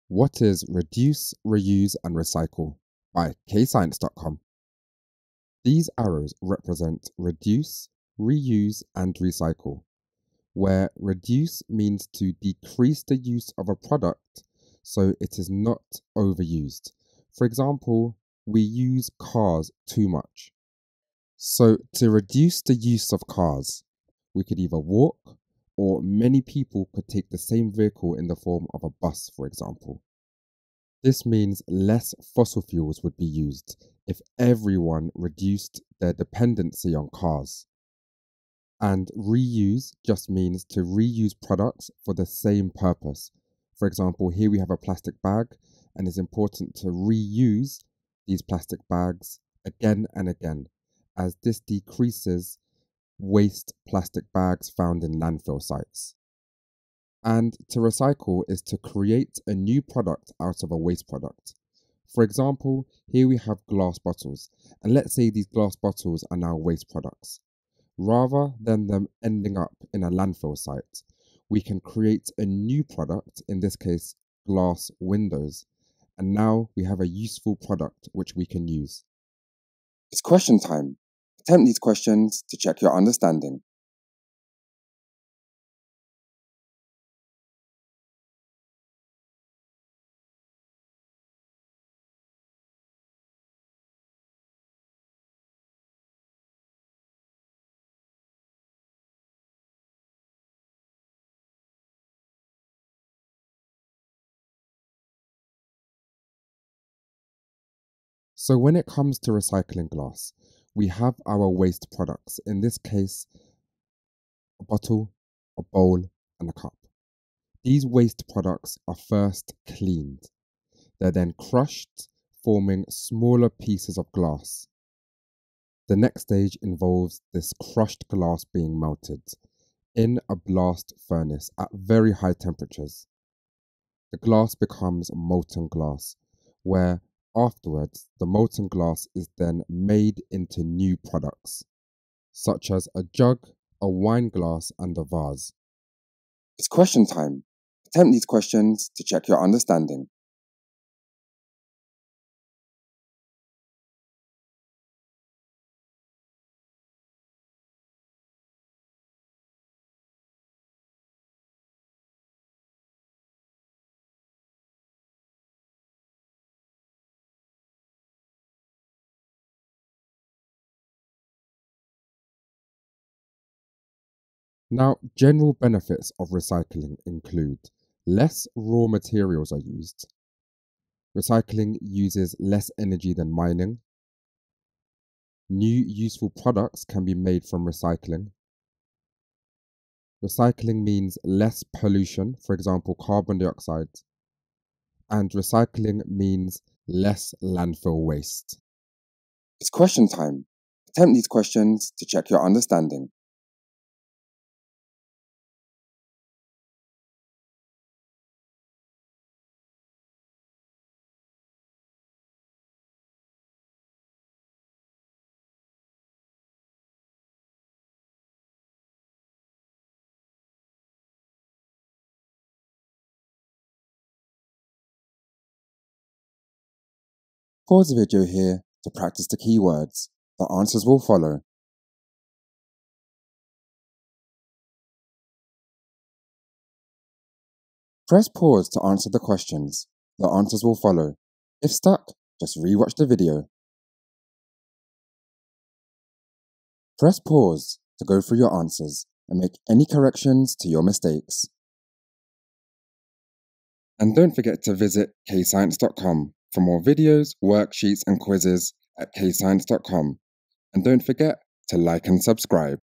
GCSE Chemistry - Reduce, Reuse & Recycle
Reduce is to decrease the use of a product so it is not overused. Reuse means reusing products for the same purpose. Recycle is to create a new product out of a waste product. When glass is recycled it must be cleaned, crushed, melted and then moulded into new glass products. Benefits of recycling includes less raw materials used, uses less energy than mining, new useful products can be made, less pollution e.g. carbon dioxide & less landfill waste.